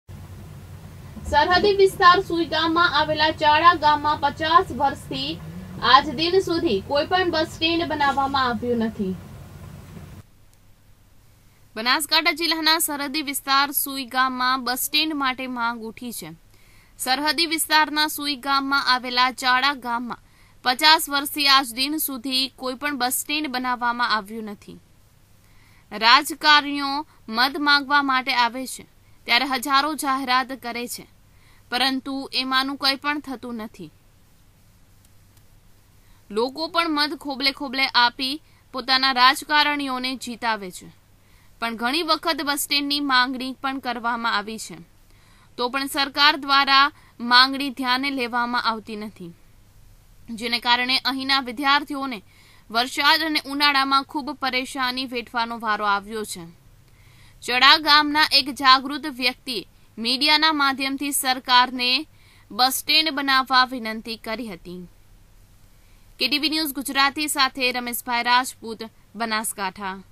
સુઇગામ સરહદી વિસ્તારના સુઇગામમાં આવેલા ચાળા ગામમાં પચાસ વર્ષથી આજદિન
સુઇગામ સરહદી વિસ્તારના સુઇગામમાં આવેલા ચાળા ગામમાં પચાસ વર્ષથી આજદિન સુધી કોઇ પણ બસસ્ટેન્ડ્ બનાવામાં આવ્યુ નથી